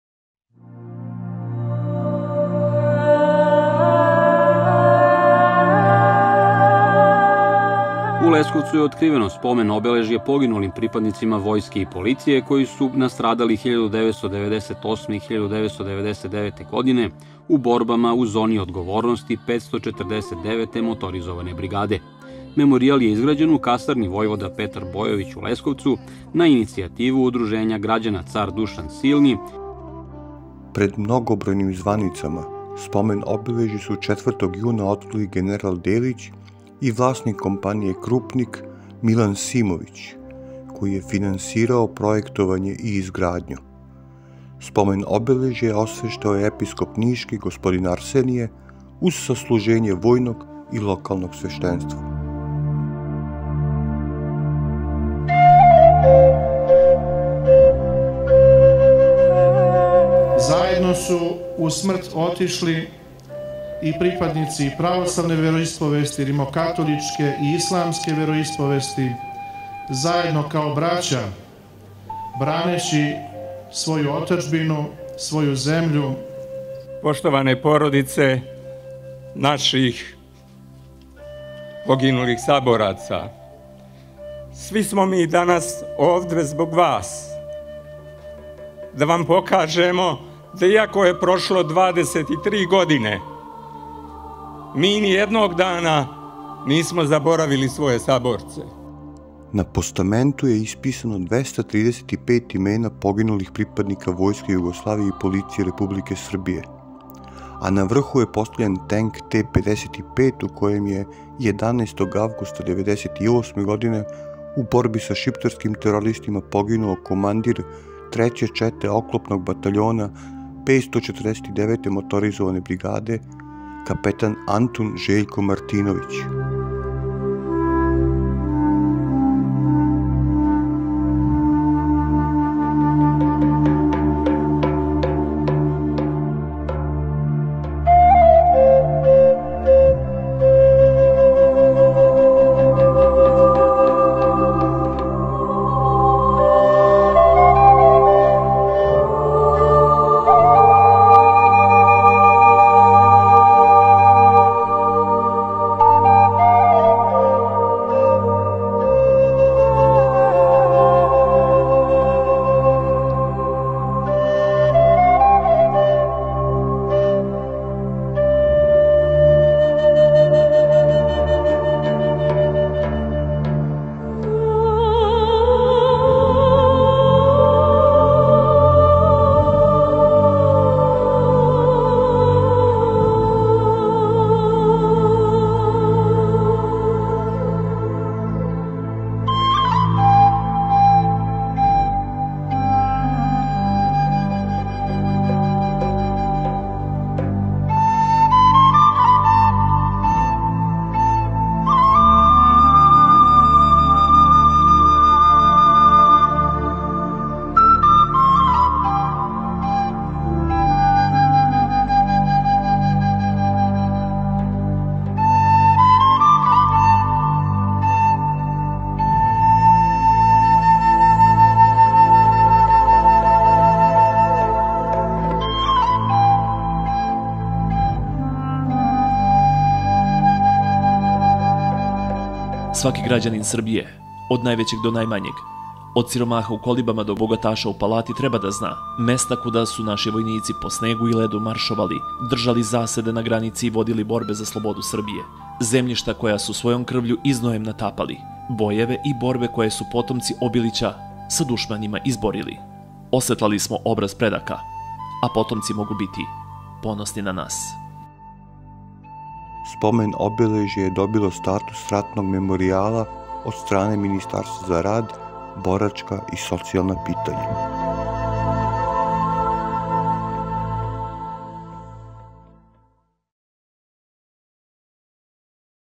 СПОМЕНИК У ЛЕСКОВЦУ 2022. / War memorial at "Field Marshal Petar Bojović" Barracks in Leskovac
На иницијативу Удружења Херојска 549. моторизована бригада „Цар Душан Силни“ , а уз подршку Министарства одбране и Војске Србије, у лесковачкој касарни „Војвода Петар Бојовић“ изграђено је Спомен обележје посвећено припадницима Војске и Полиције који су 1998. и 1999. године погинули у борбама са шиптарским терористима и снагама НАТО у Зони одговорности 549. МтБр (општине Призрен, Ораховац, Сува Река, Ђаковица и Гора).
На монументалном постаменту Спомен обележја исписано је 235 имена погинулих припадника Војске Југославије и Полиције Републике Србије у Зони одговорности 549. МтБр.
* 103 имена припадника 549. МтБр;
*  36 имена припадника из јединица Приштинског корпуса;
*  6 имена припадника јединица Нишког корпуса;
*  6 имена припадника 63. падобранске бригаде;
*  6 имена припадника 72. специјалне бригаде;
*  76 имена припадника из састава Полиције Републике Србије и
*  2 имена руских добровољаца који су се борили у саставу српских снага безбедности.
На врху постамента је постављен тенк Т-55 у којем је 11. августа 1998. године у борби са шиптарским терористима погинуо командир 3. чете оклопног батаљона 549. МтБр капетан Антун Жељко Мартиновић.